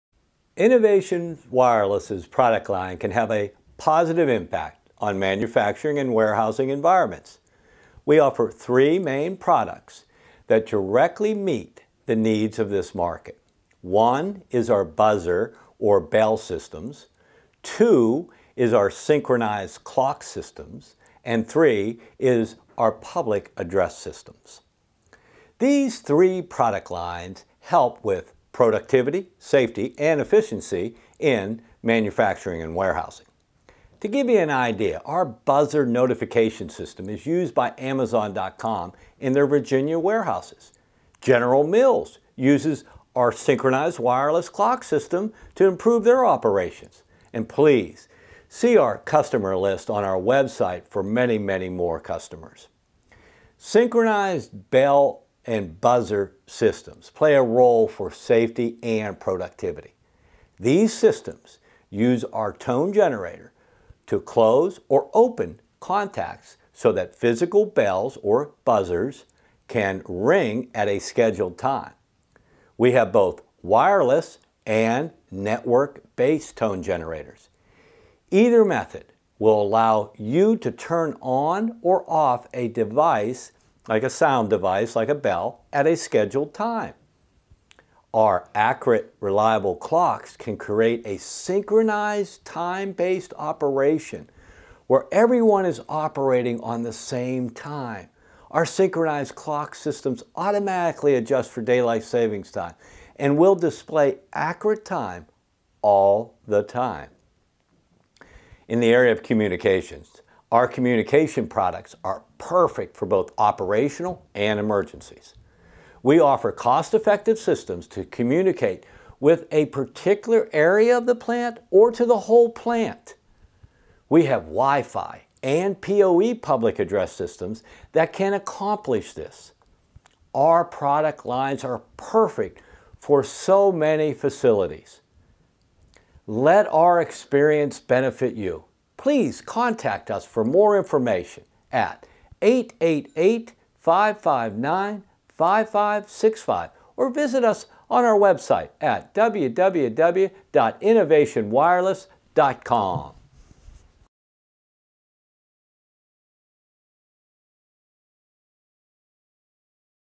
Industrial Clocks and Communication Systems
Clock and Buzzer Systems are Crucial to Industrial Facilities. We offer reliable, accurate, maintenance free clock and alert systems.

Innovation Wireless' product line can have a positive impact on manufacturing and warehousing environments.

We offer three main products that directly meet the needs of this market. One is our buzzer or bell systems. Two is our synchronized clock systems and three is our public address systems.

These three product lines help with productivity, safety and efficiency in manufacturing and warehousing. Please take a Look at our tone generators at:

To give you an ideal our buzzer notification system is used by Amazon.com in their Virginia Warehouses.  General Mills uses our synchronized wireless clock system to improve their operations.

In the area of communications are communication
products are perfect for both operational and emergencies. We offer cost effective systems to communicate a particular area of the plant or to the whole plant.

We have Wi-Fi and PoE public address systems that can accomplish this. Our product lines are perfect for so many facilities. Let our experience benefit you.